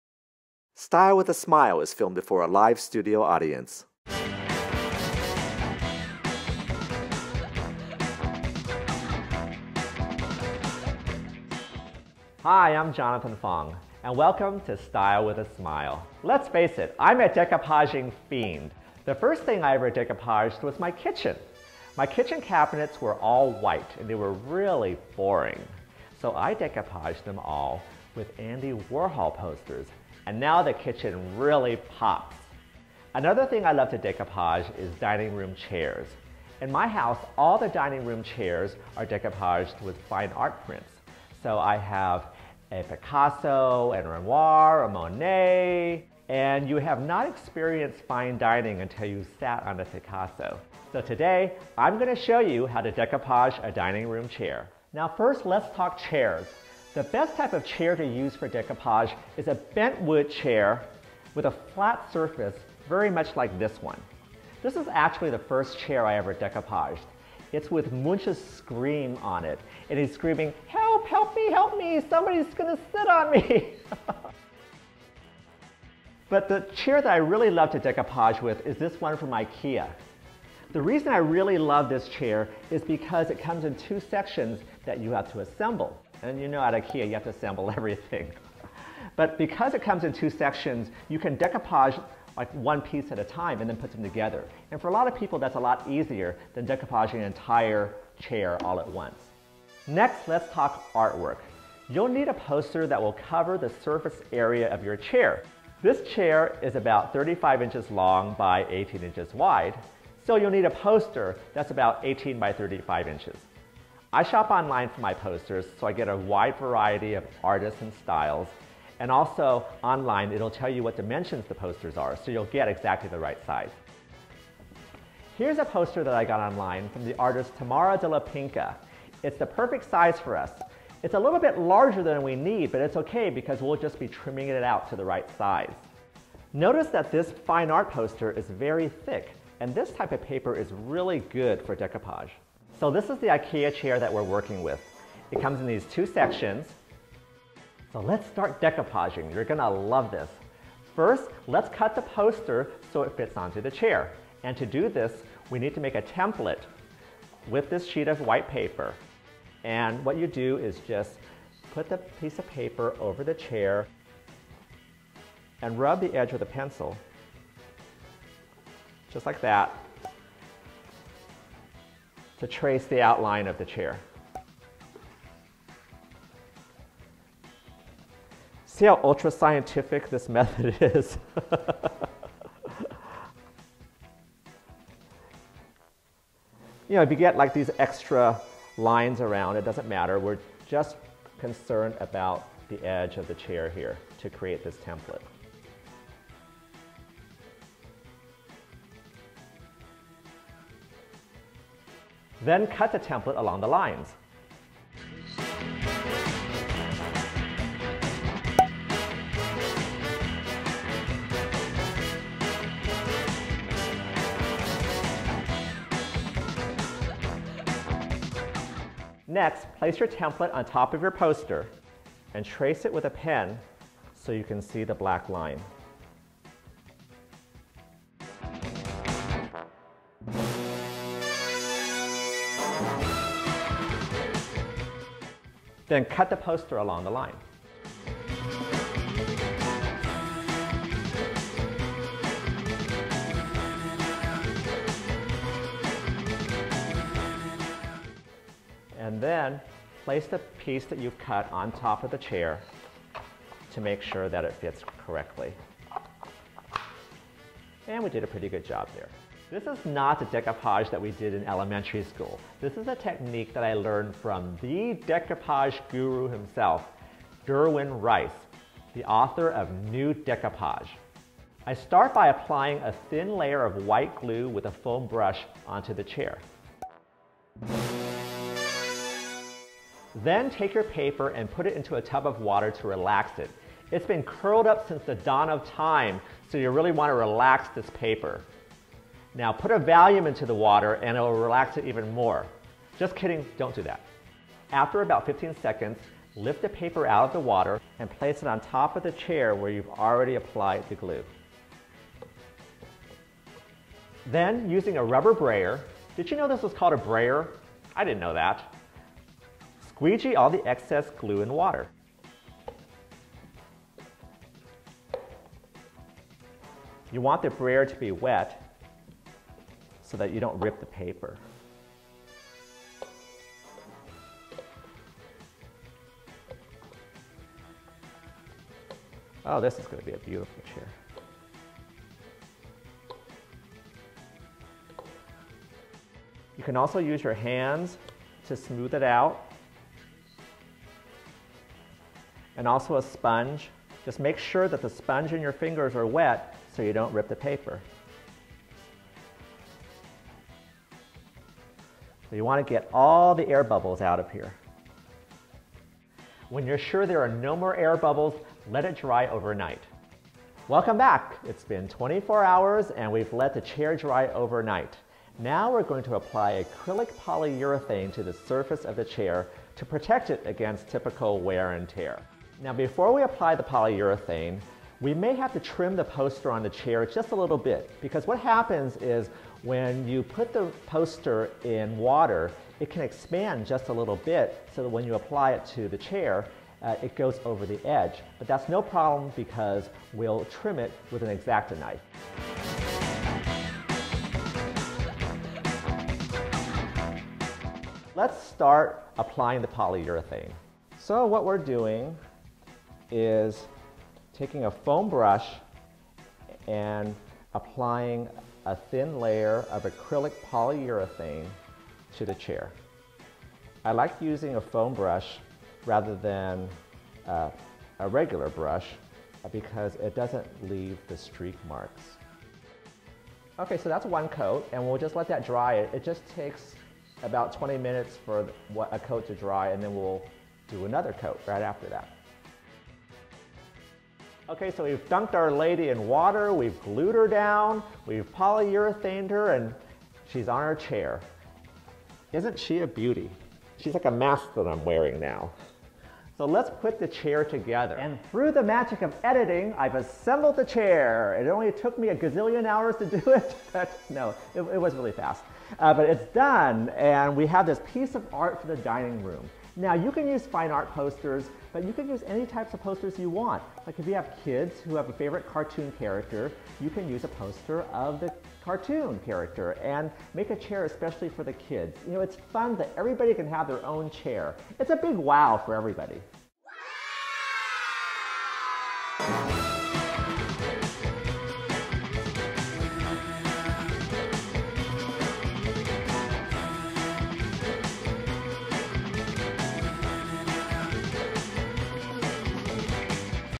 Decoupage Chairs
Create a furniture piece worthy of a gallery, as Jonathan Fong shows you how to decoupage fine art posters onto dining room chairs.